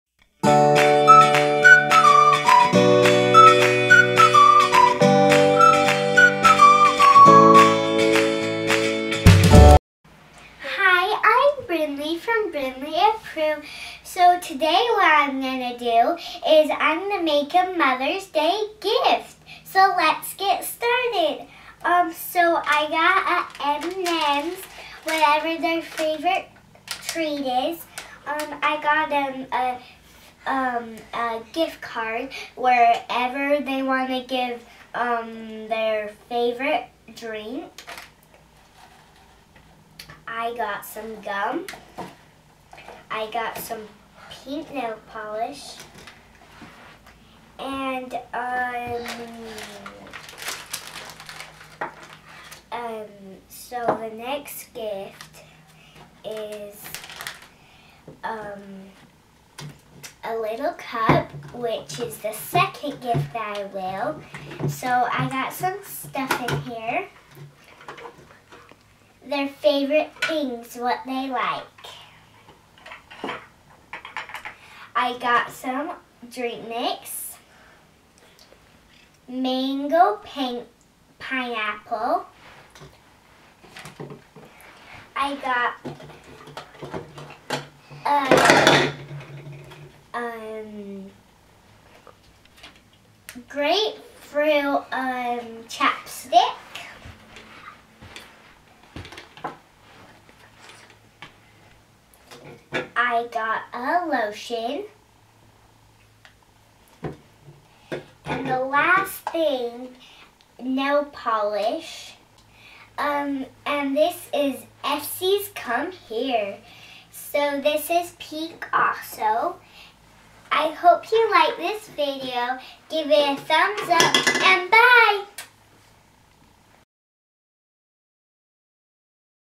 Mother's Day Gift Guide / Brynlee Approved
Hey Guys this is an easy DIY Mother's Day Gift Guide.
The first gift we found a cute Butterfly container and filled it with some of our Moms/Grandma's favorite things: M&M's, Gum, Gift Card (Jamba Juice), and Nail Polish (Sinful Colors: Thimbleberry)
The Second gift we found a really fun Summer time Mason Jar Cup with Lemons and Lemonade on it. Filled it with Moms/Grandma's favorite things: Burts Bees Grapefruit Chapstick, Drink Mix (Mango Pineapple), Nail Polish (Essie: Come Here), Hand Lotion (Mango Mint from Ulta)